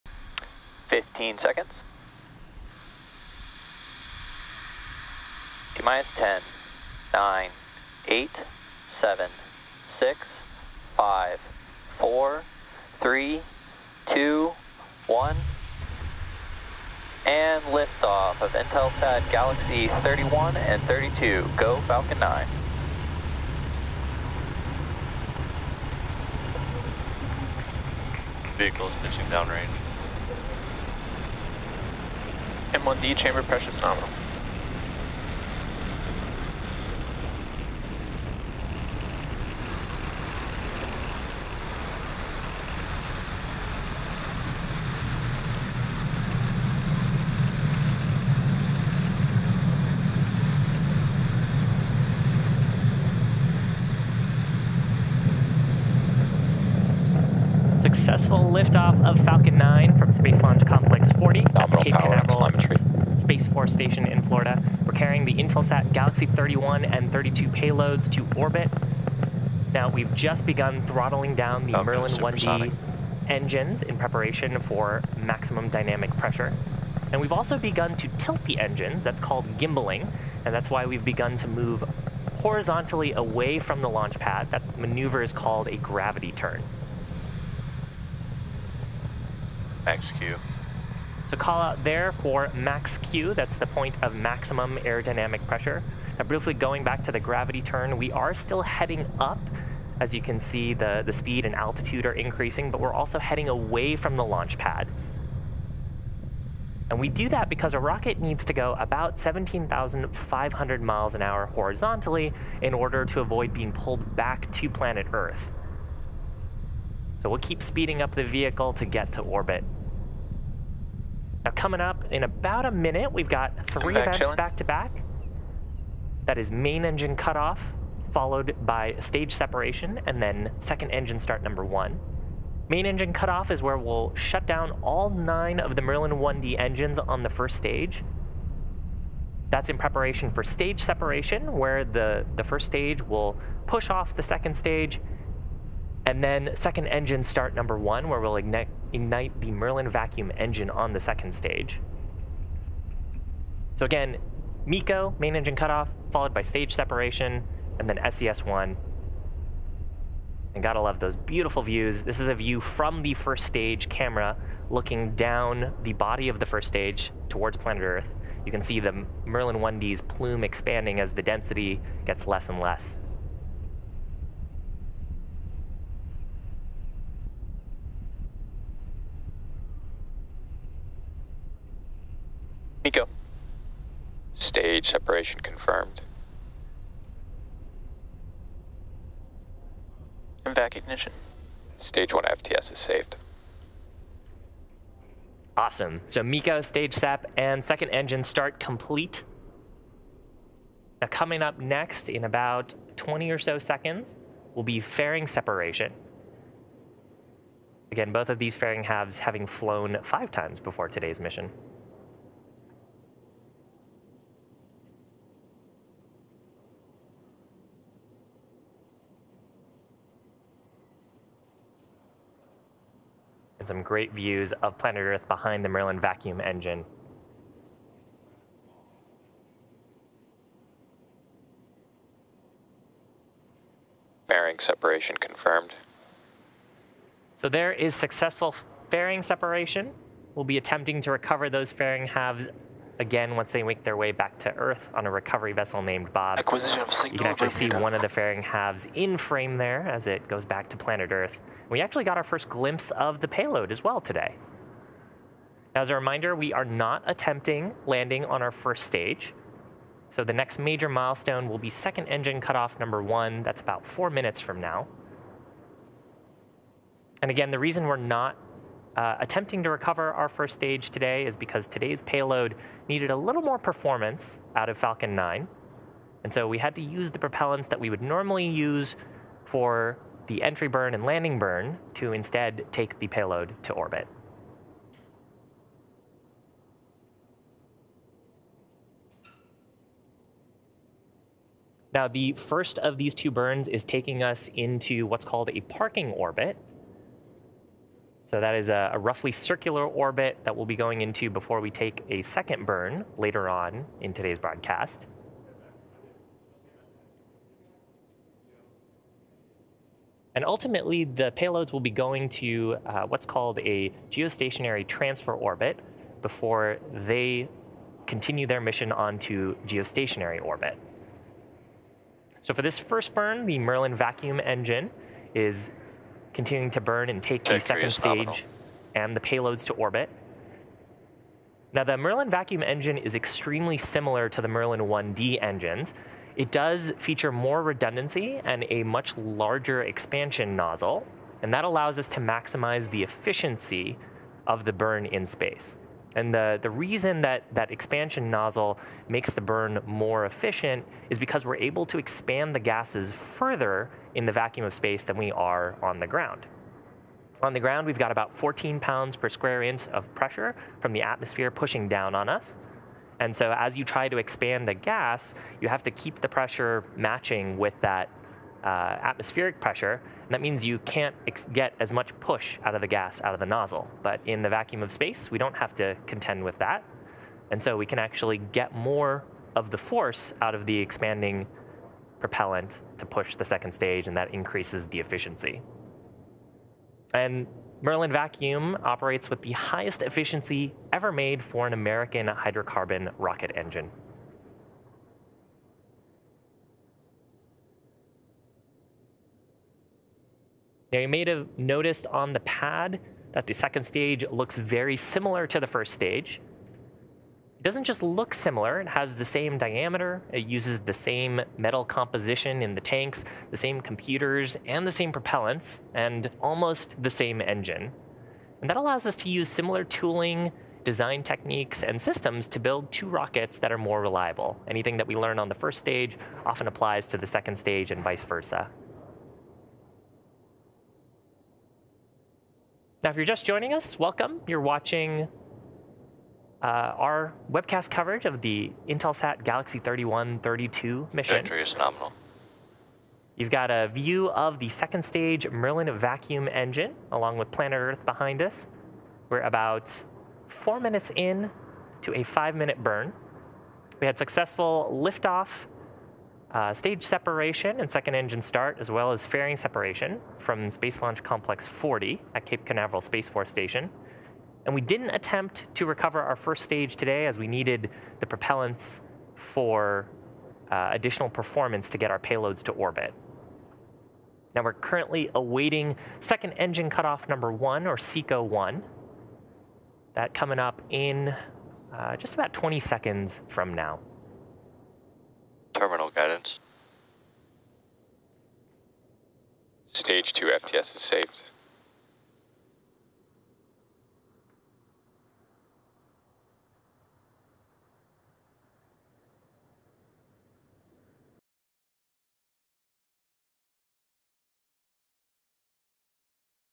Watch SpaceX Falcon 9 Rocket Launch (Intelsat G-31/G-32 Mission)
Watch Falcon 9 launch the Intelsat G-31/G-32 mission from Space Launch Complex 40 at Cape Canaveral Space Force Station in Florida.